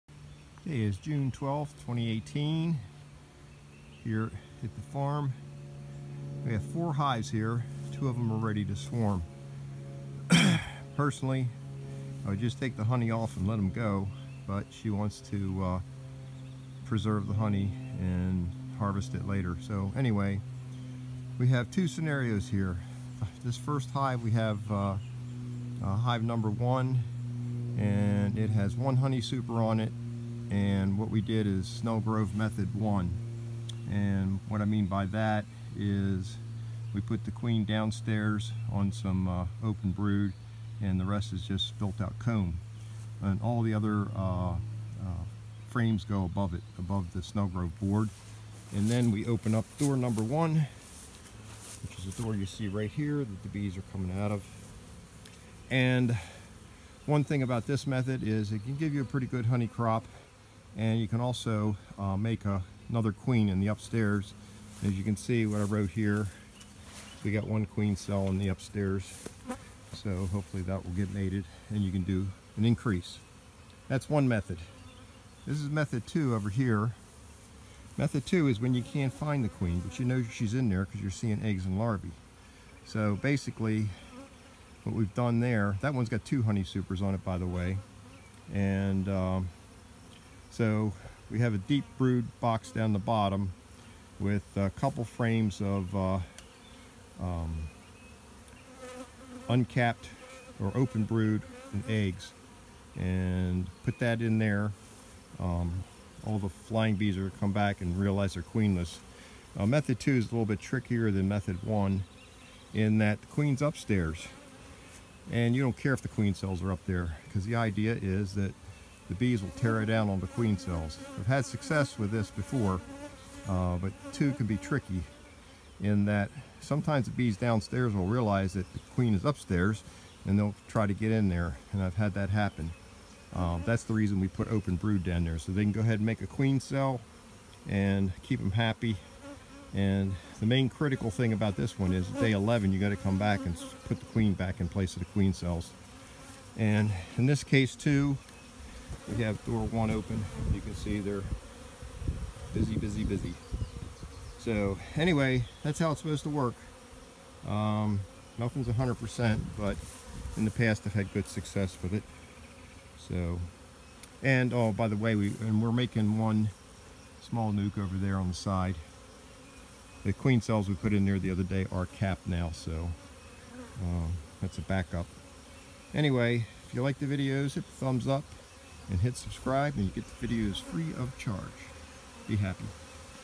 Snelgrove Method 1 & Method 2 modified
Here is a brief description of two Snelgrove swarm control methods side by side. Method 1 is on Hive 1 and is the most used Snelgrove method. The queen is in the bottom box with the frame she was on and the rest open comb. The SG board separates the flying bees and queen from the house bee. Method 2 is used when you have advanced queen cells and it is not necessary to find the queen.